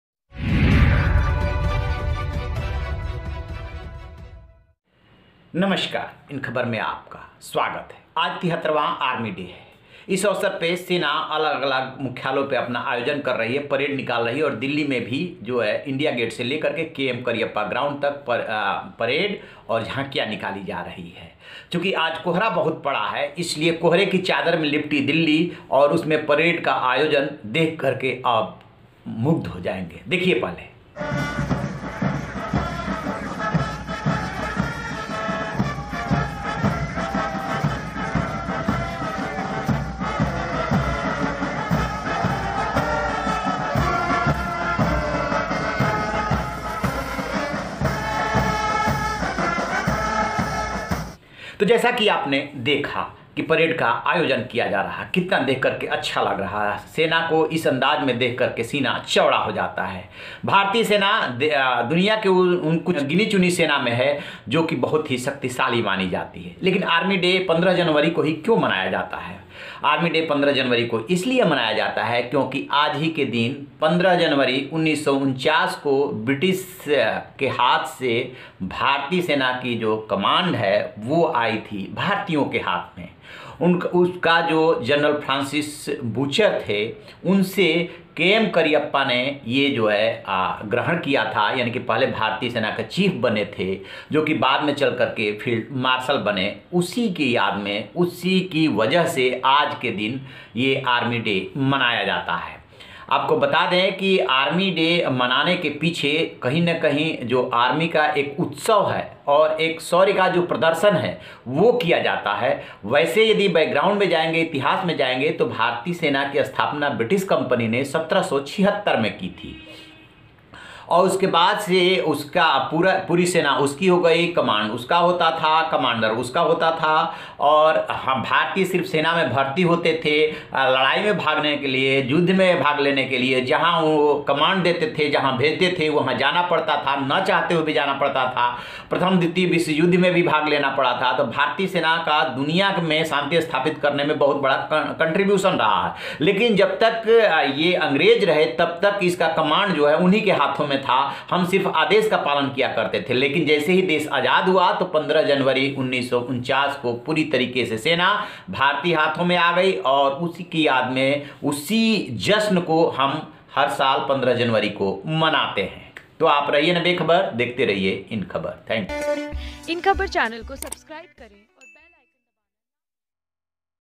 Army Day 2021: Why Army Day is celebrated on 15th January, 15 जनवरी को क्यों मनाते हैं सेना दिवस
भारतीय सेना अपना 73वां Army Day 2021 मना रही है,  देश में हर साल 15 जनवरी को भारतीय थल सेना दिवस मनाया जाता है, क्यों मनाते हैं आर्मी डे जानिए विस्तार से.